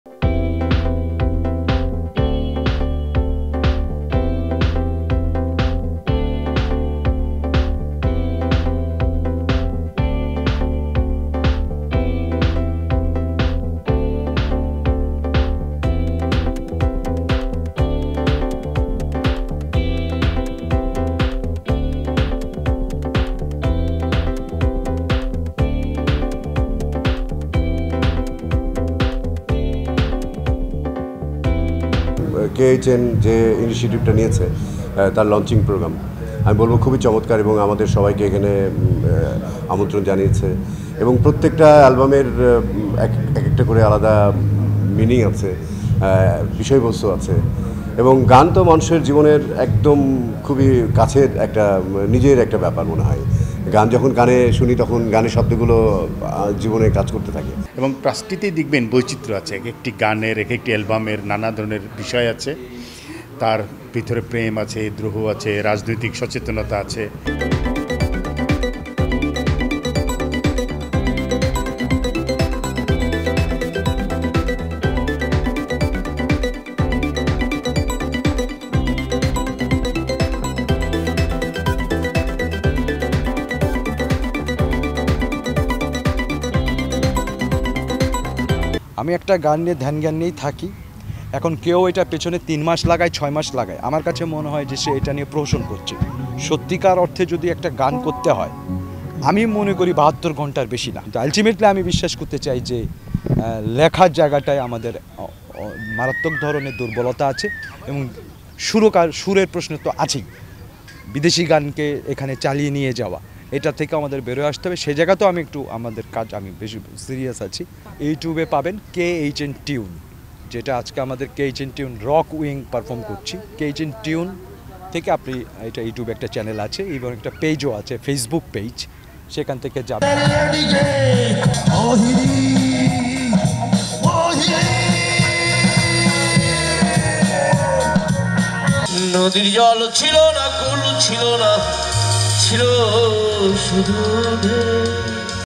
৫টি একক গানের অ্যালবাম প্রকাশ করলেন কামরুল হাসান নাসিম | News24
৫টি একক গানের অ্যালবাম প্রকাশ করলেন কামরুল হাসান নাসিম | News24

NEWS24 Team